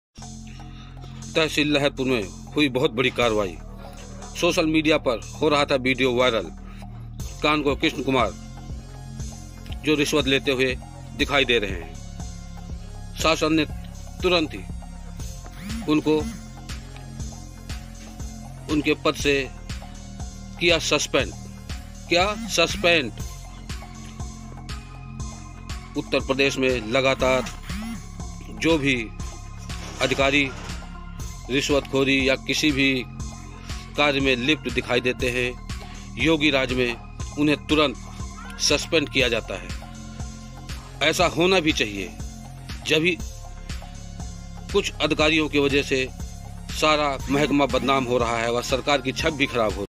April 2, 2022
तहसील लहरपुर में किसको किया गया सस्पेंड रिश्वत लेते हुए हुआ था वीडियो वायरल।